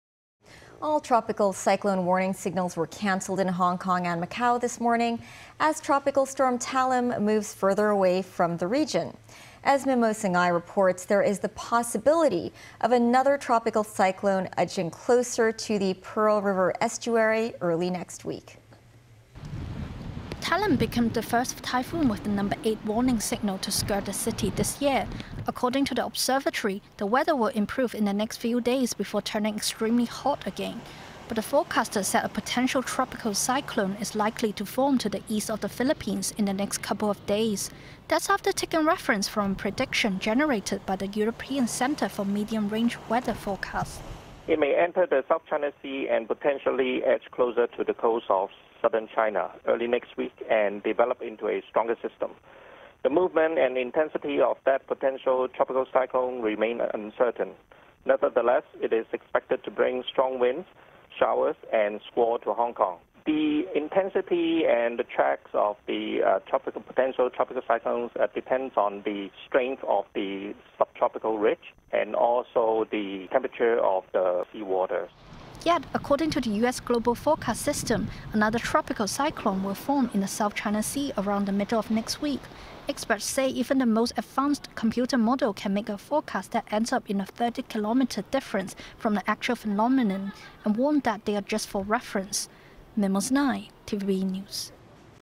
TVB News | Report | 18 Jul 2023 | Another tropical storm possible next week
As Tropical Storm Talim moves further away from the region, weather forecasters say there is the possibility of another tropical cyclone heading this way next week.